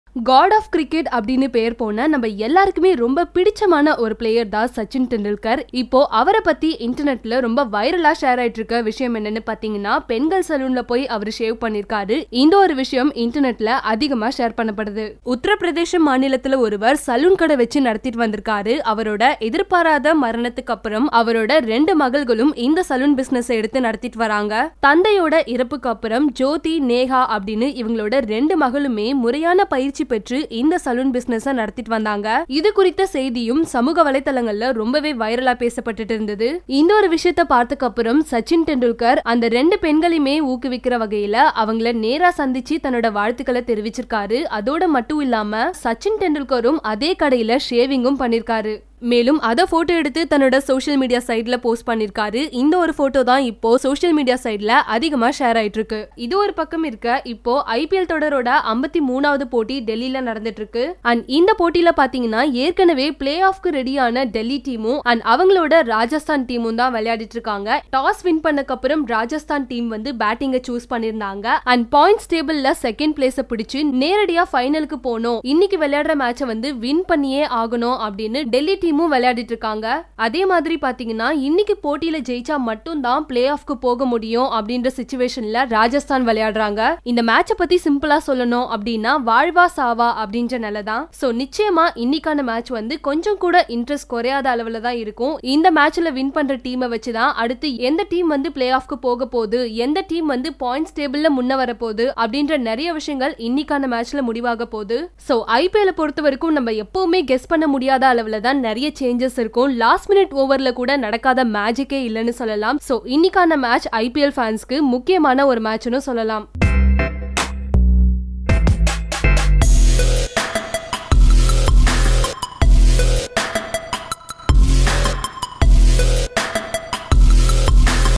Sachin Tendulkar Gets A Shave From ' Barbershop Girls ' | Latest News
Legendary cricketer, Sachin Tendulkar recently got a shave from a woman. The cricketer did this to show his support towards women barber and said that this was the first time he got a shave from someone else.